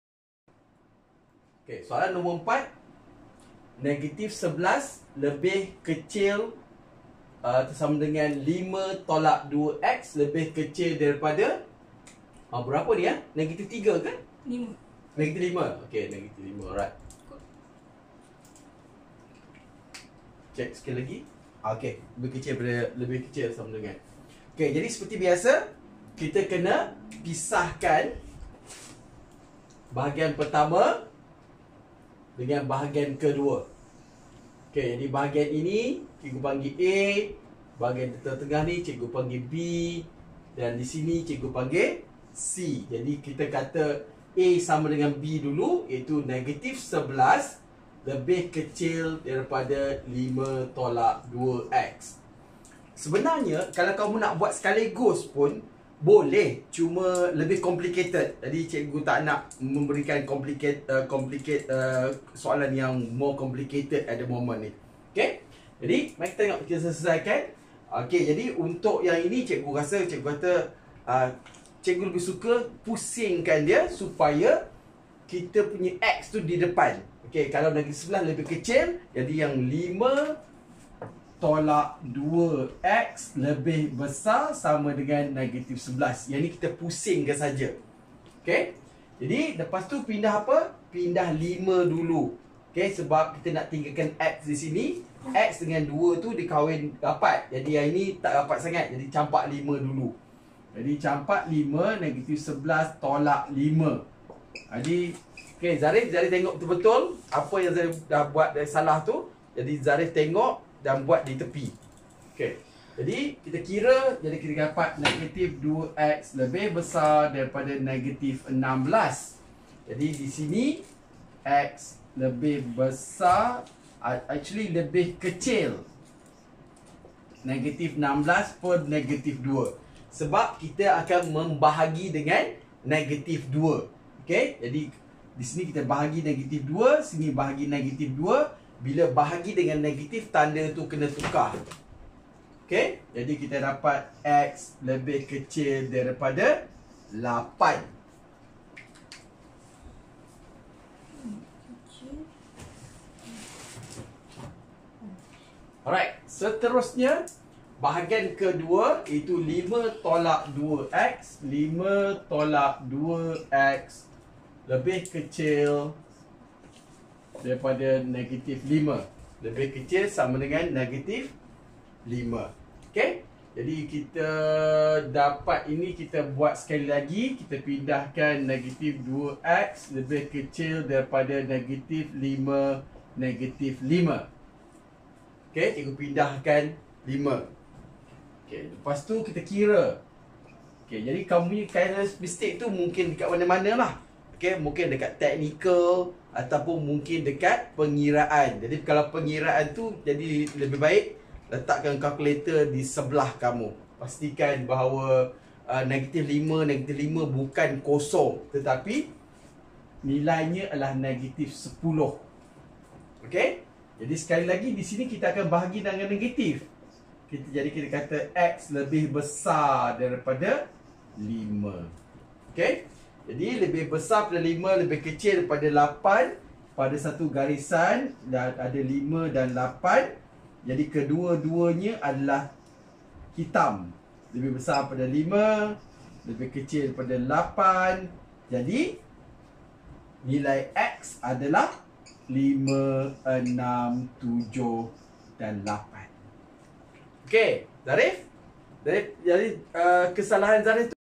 Bagaimana mendapatkan julat nilai integer yang memenuhi ketaksamaan linear berikut -11≤5-2x≤-5
Soalan ini didapati daripada buku Oxford Fajar SUCCESS Matematik SPM 2019, pada muka surat P27, soalan 4, ketaksamaan linear.

Bagaimana mendapatkan nilai nilai integer yang memenuhi ketaksamaan berikut -11≤5-2x≤-5